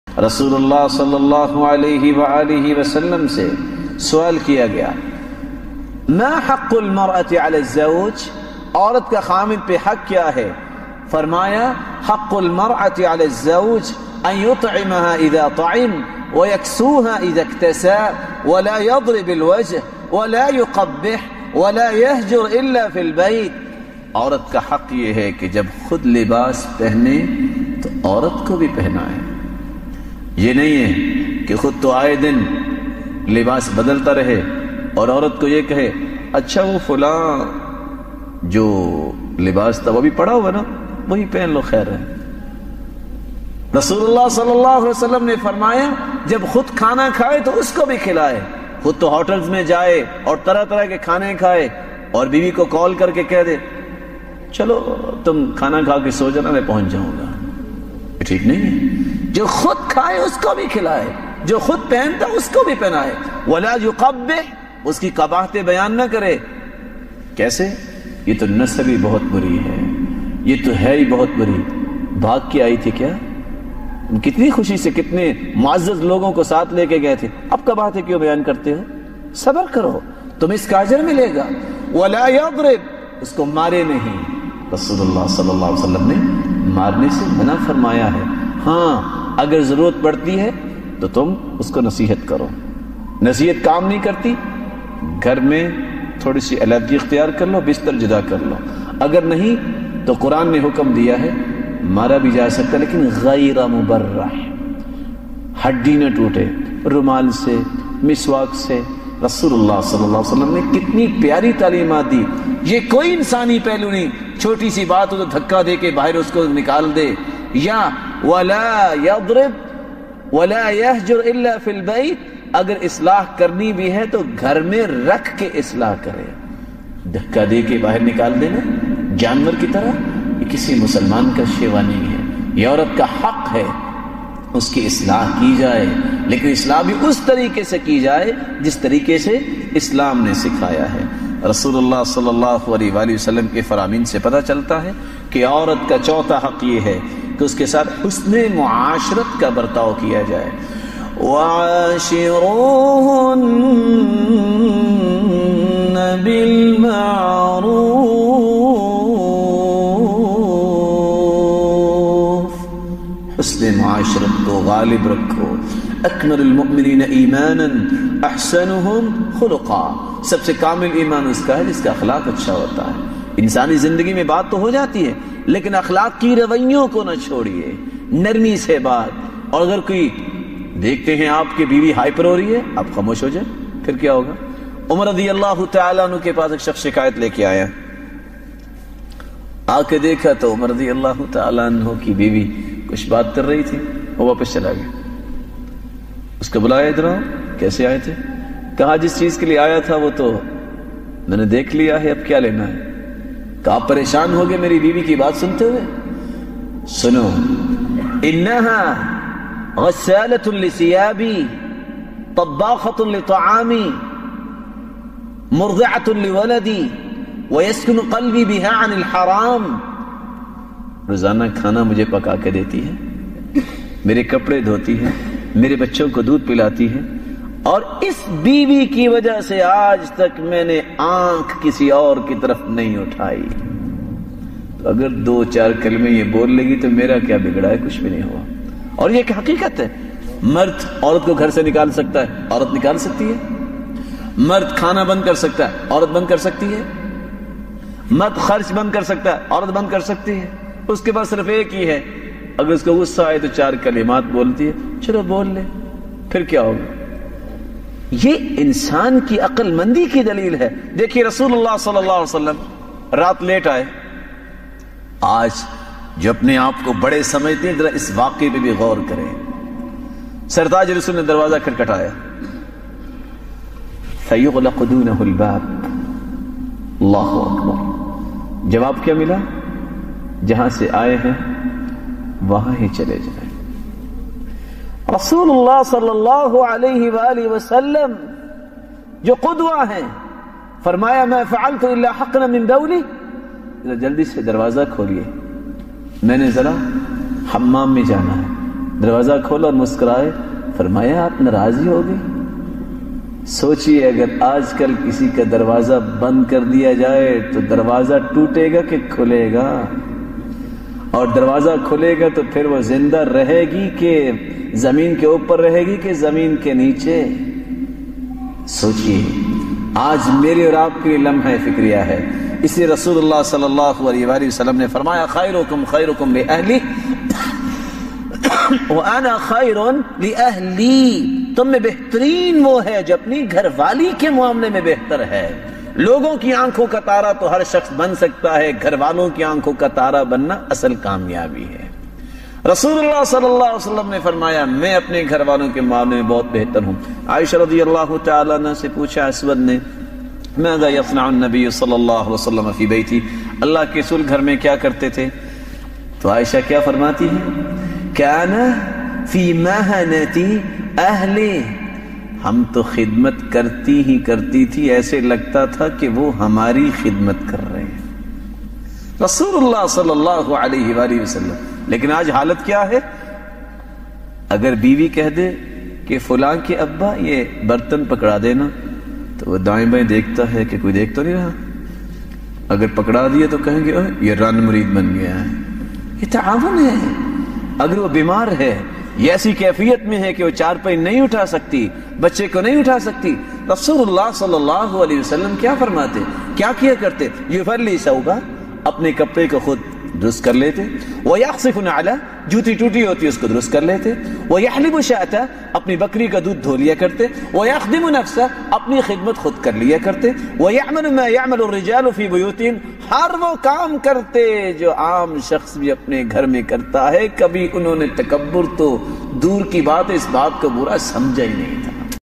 Miya Biwi Ke Huqooq || Part 9 || میاں بیوی کے حقوق || Fazilatul Sheikh Qari Sohaib Ahmed
📚 میاں بیوی کے حقوق || نواں حصہ

🌺 فضیلتہ الشیخ قاری صہیب احمد میر محمدی حافظہ اللہ

📚 Miya Biwi Ke Huqooq || Part 9

🌺 Fazilatul Sheikh Qari Sohaib Ahmed Meer Mohammadi Hafizahullah

واٹس ایپ پر اسلامک میسیجیس حاصل کرنے کیلئے اپنا نام لکھ کر اس نمبر پر واٹس ایپ کریں